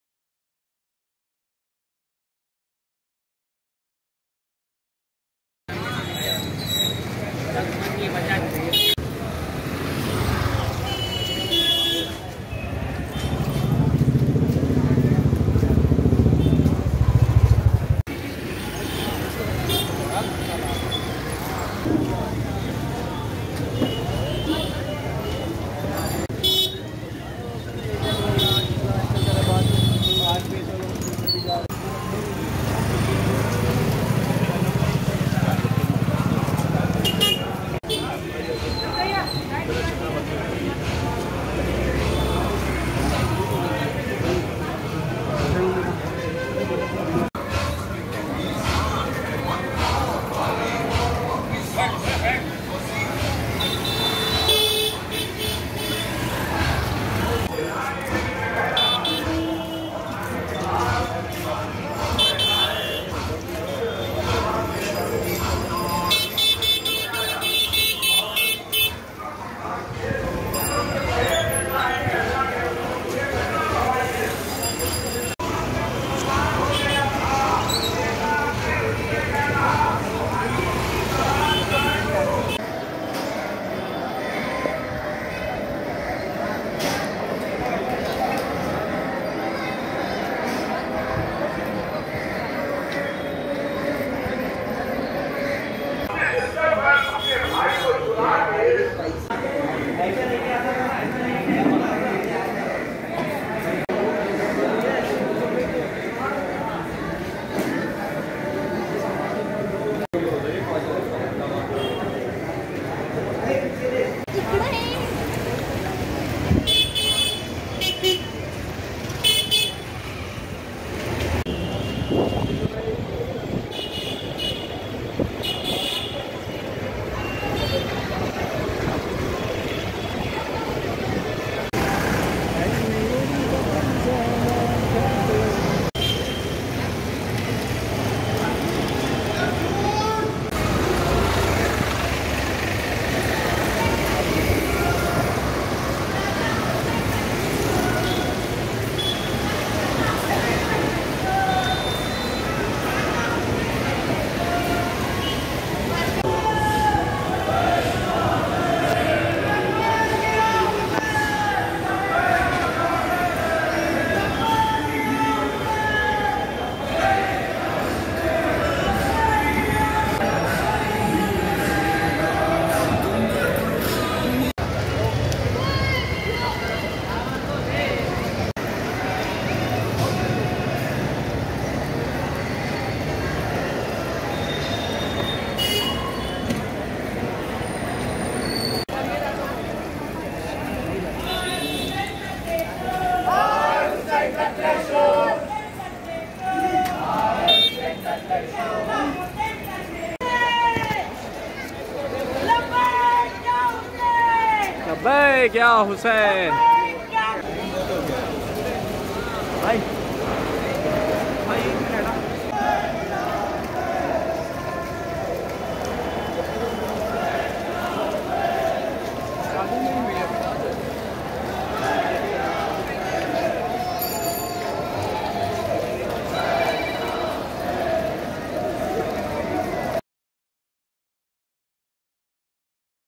MUMBAI | INTEZAAM | JULOOS | SHAB E ASHURA 2023
Shab e Ashura (Martyrdom of Hazrat Imam Husain (A.S) and Shohadaye Karbala (A.S)) was Commemorate in Mumbai, Maharashtra, India on 28th of July 2023.
More than 05 (Five) Lakhs people participated in Shab e Ashura and Ashura Juloos (Procession) from different places of Mumbai and several parts of Maharshtra.
The Shab e Ashura procession Starts from Imambada Hazrat Abbas (A.S) byculla and ends at Imamia Masjid, Imambada Road, Bhendi Bazar.
The Ashura procession starts from Zainabia Imambada, Moghul Masjid, Imamia Masjid (Bhendi Bazar) and Khoja Masjid(Dongri) ends at Rehmatabad Cemetry (Qabrastan), Mazgaon. Via. Noor Baug.
The Ashura juloos is largest juloos held in Mumbai and also called as Markazi juloos thats covers  approx. 06(Six) Kilo meters distance. People (Azadars) participate in this juloos without wearing their footwears.